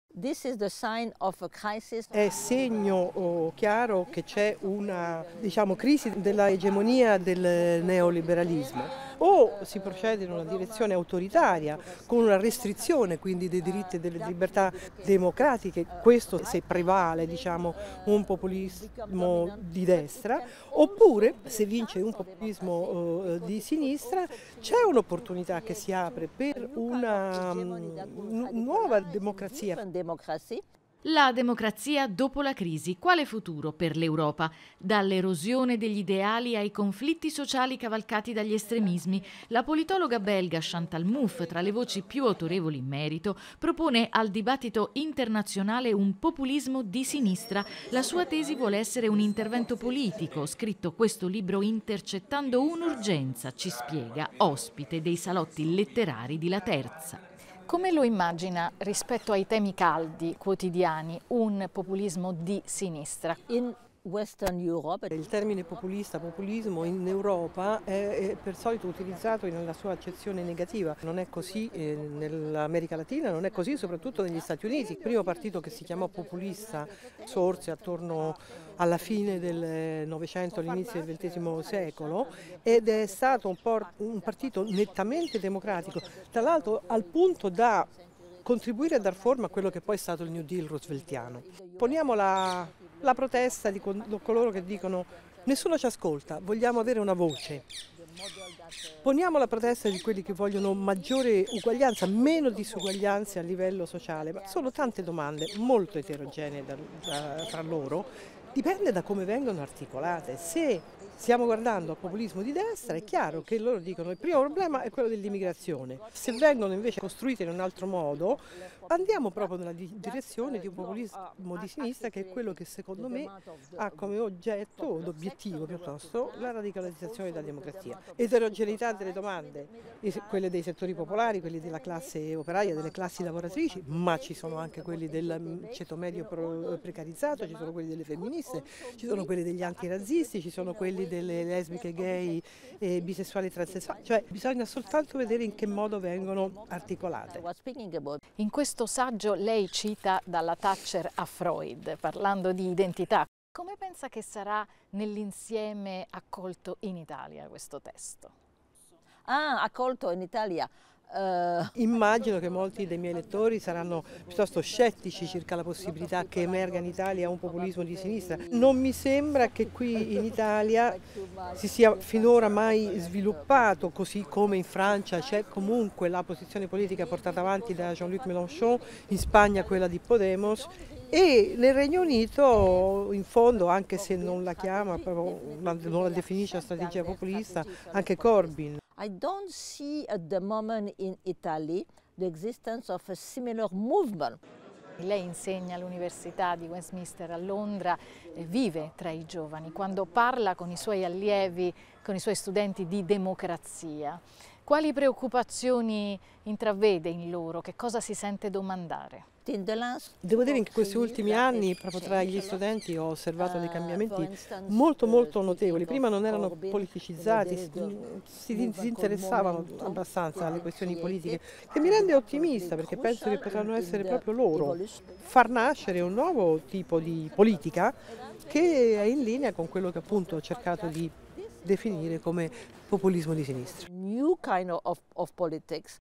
Intervista a Chantal Mouffe, autrice di "Per un populismo di sinistra"
L'intervista a Chantal Mouffe, autrice di "Per un populismo di sinistra", di Loretta Cavaricci, andata in onda il 17/02/2019 su Rainews.